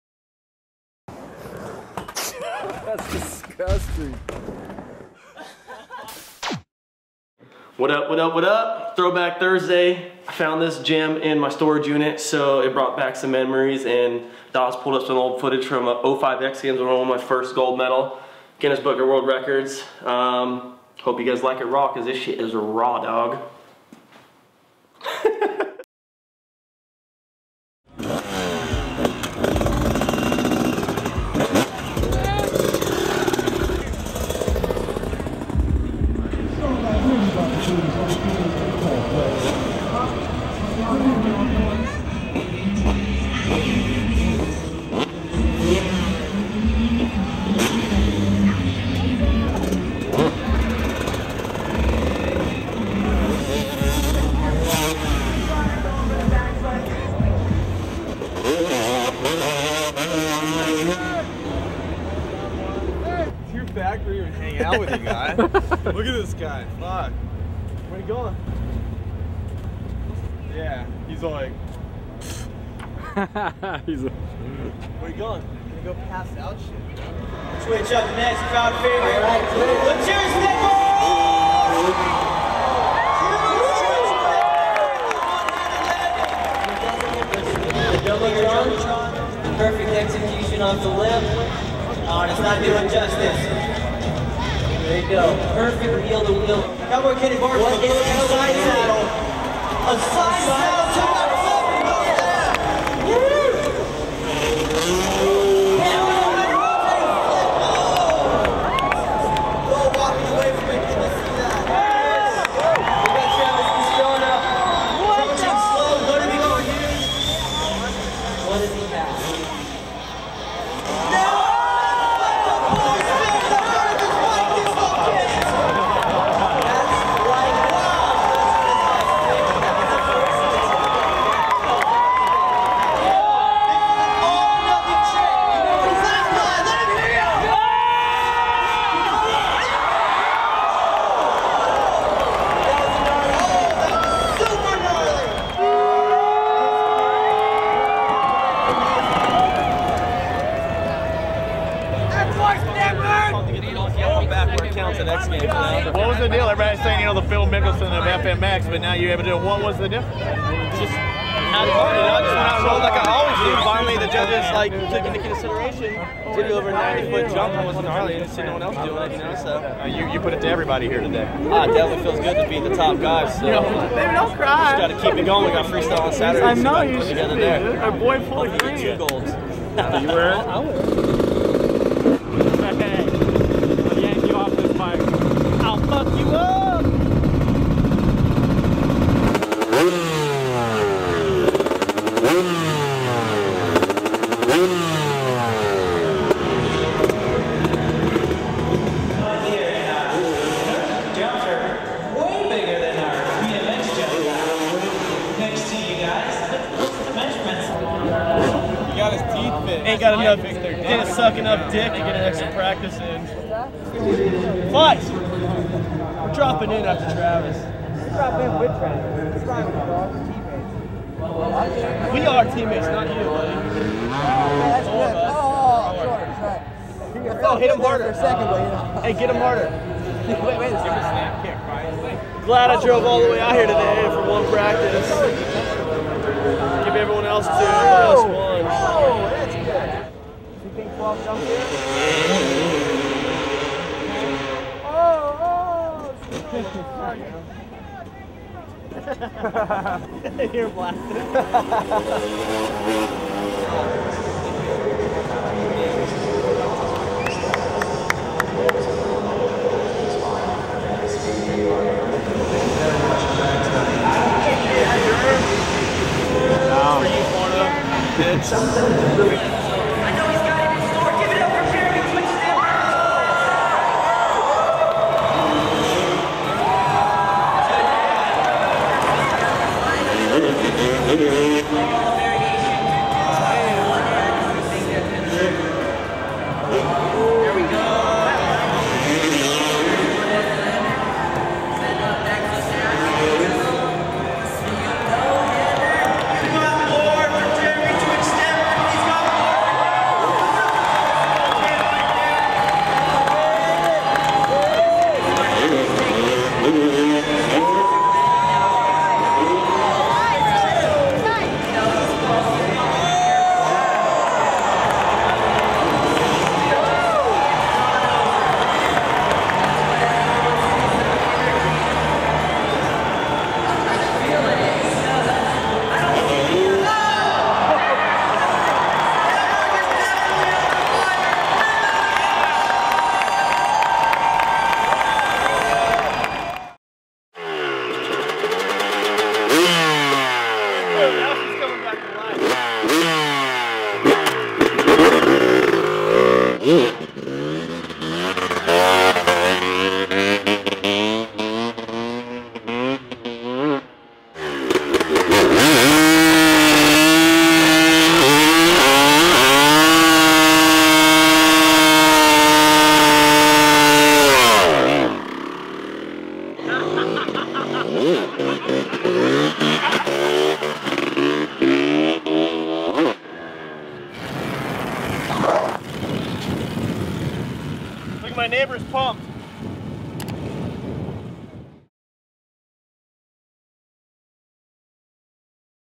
Twitch & Pastrana Battle for Gold
Another Throwback vid from the vault! We broke out the 2005 X Games gold medal bike from storage so we also pulled some raw video from that event. Jeremy won his 1st Best Trick gold medal & also got the world record for longest backflip at the time, beating Pastrana & Nate Adams. Good memories on the two-stroke haha.

Film & Edit by Dave Dawes and J.Sanders